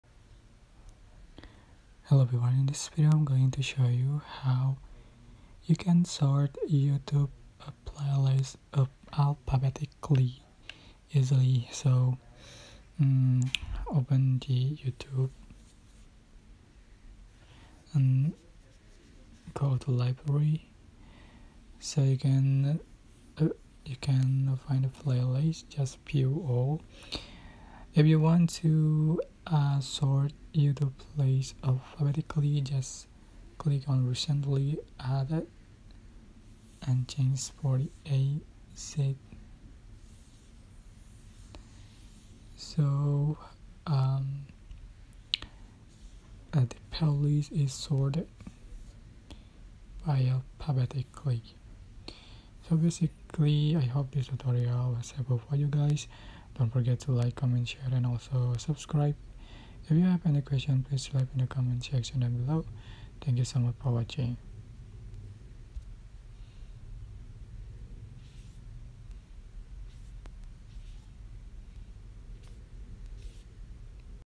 How To Sort YouTube Playlist Alphabetically? NEW UPDATE JULY 2023 | Youtube App Tutorial!
will show you How To Sort YouTube Playlist Alphabetically